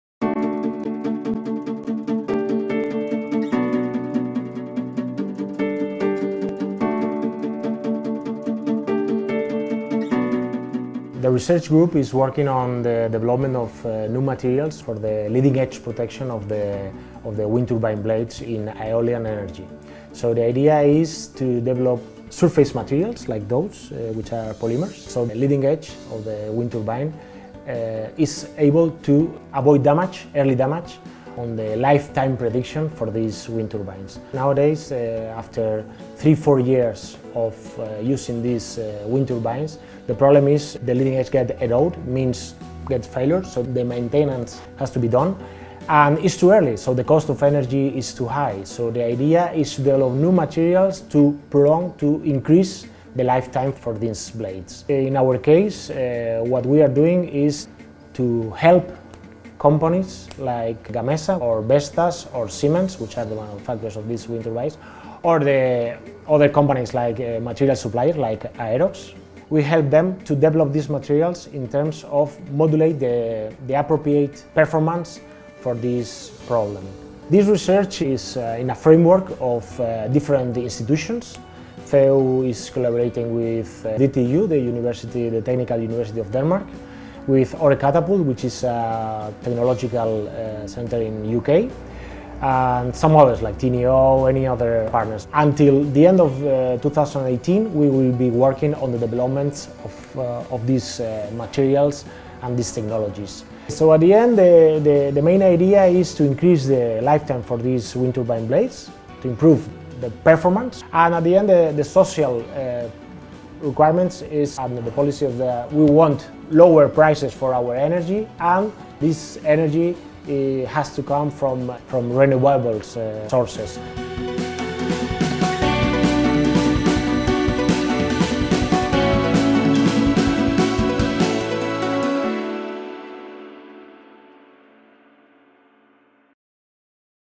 Professor Fernando Sanchez about New Material for the Eolian Energy (The Horizon 2020)
In wind energy, one of the main research challenges is the development of new wind turbine blade coating materials that improve their resistance to erosion, especially when laid on the sea. The TecEner research group at CEU UCH, led by Professor Fernando Sánchez López, is part of a European consortium with ten research centers and companies working in this field, with funding from the Horizon 2020 program from the European Unión.